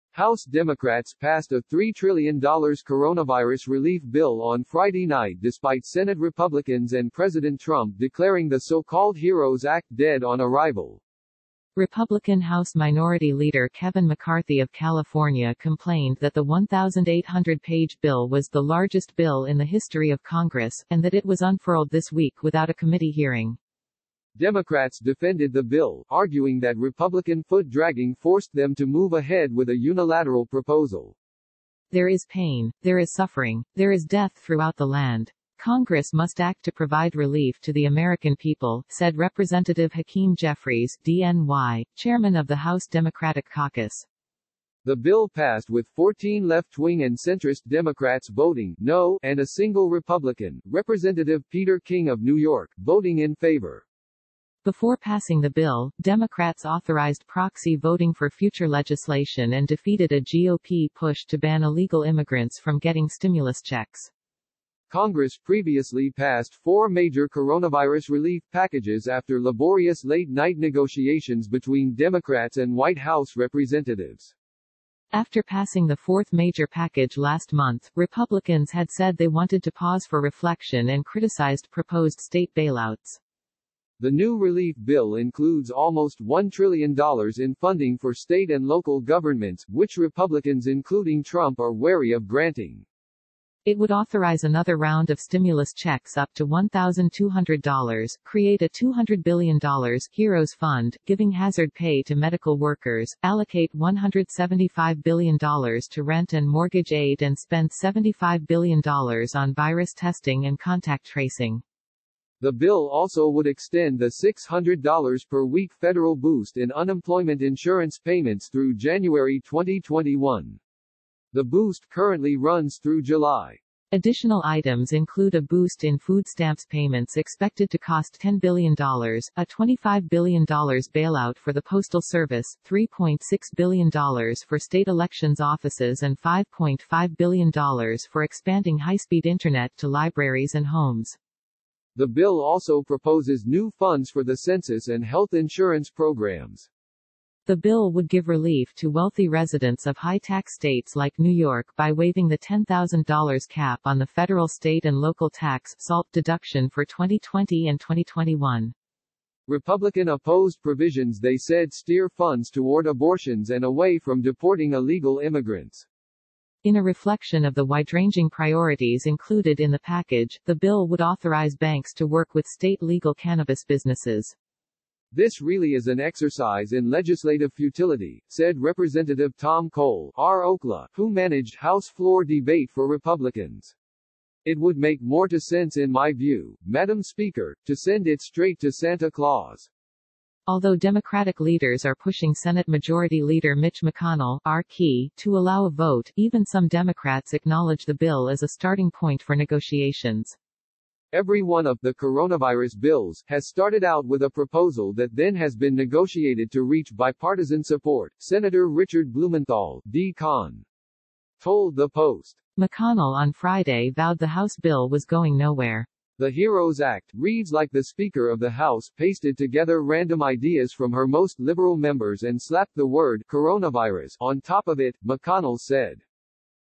House Democrats pass doomed $3T coronavirus relief bill
House Democrats passed a $3 trillion coronavirus relief bill on Friday night despite Senate Republicans and President Trump declaring the so-called Heroes Act dead on arrival.